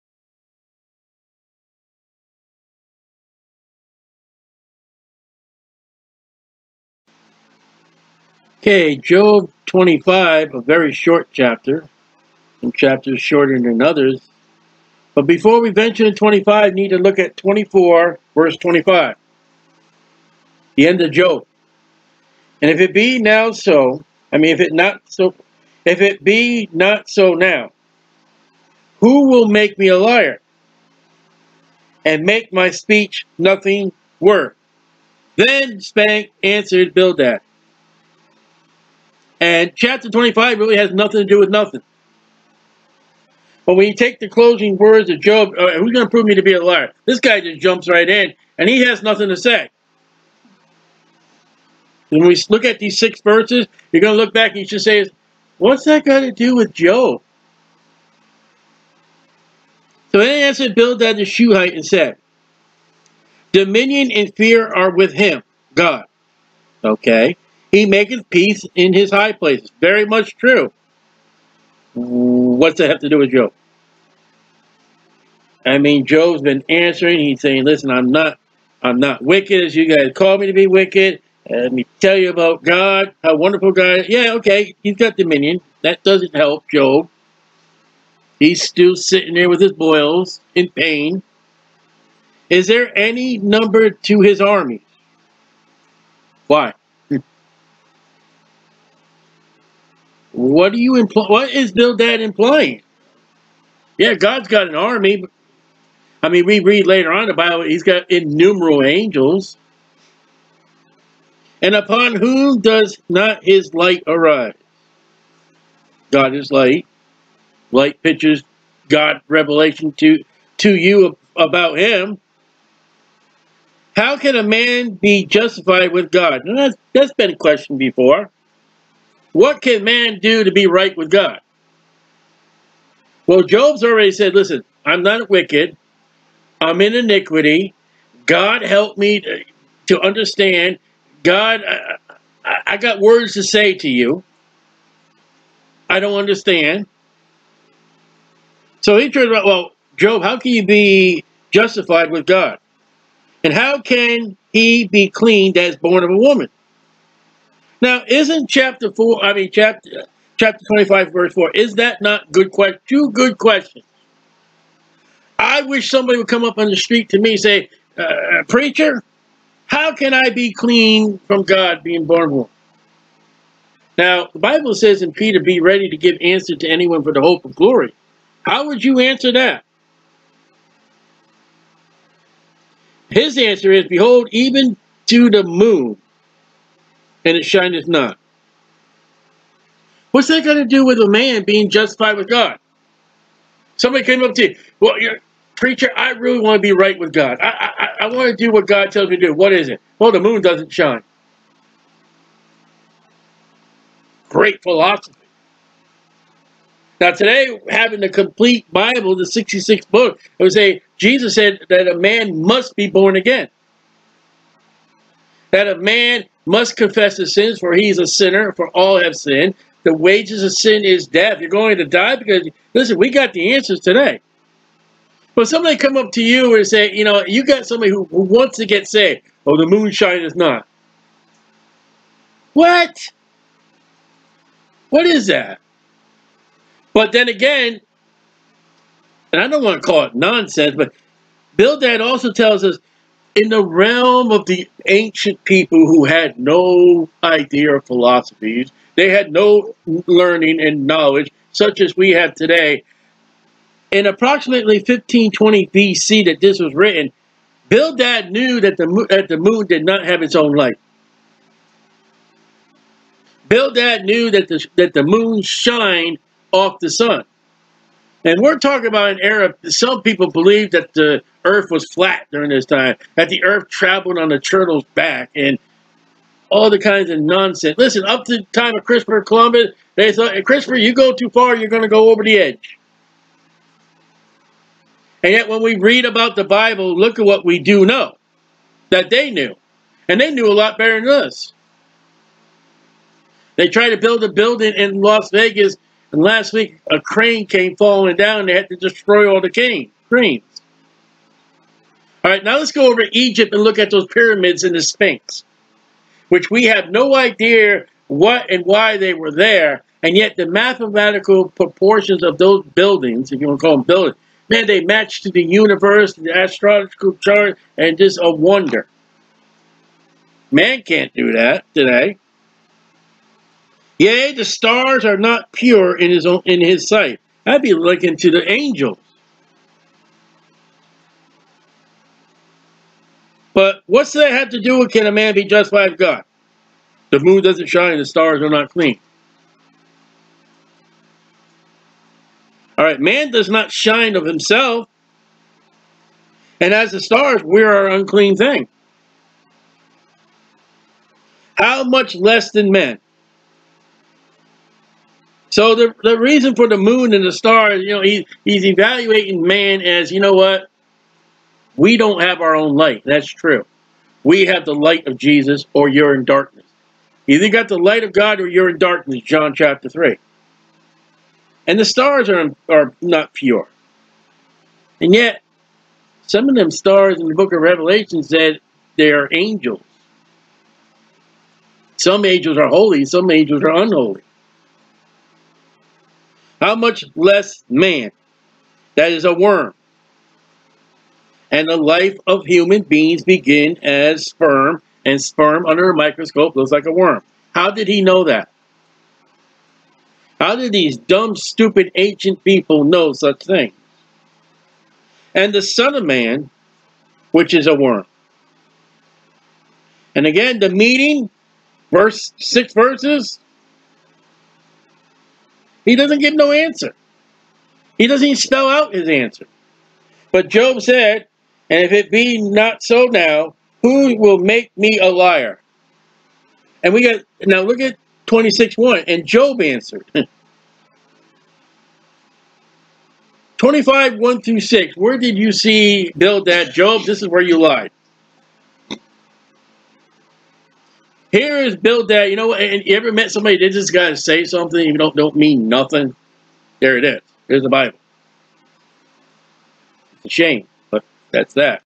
Man Job 25
What are a man and his power? To answer Job, what is the meaning of Bildad's words?

Jesus the Word.

Write To Us:
Jesus Christ
The Blessed Hope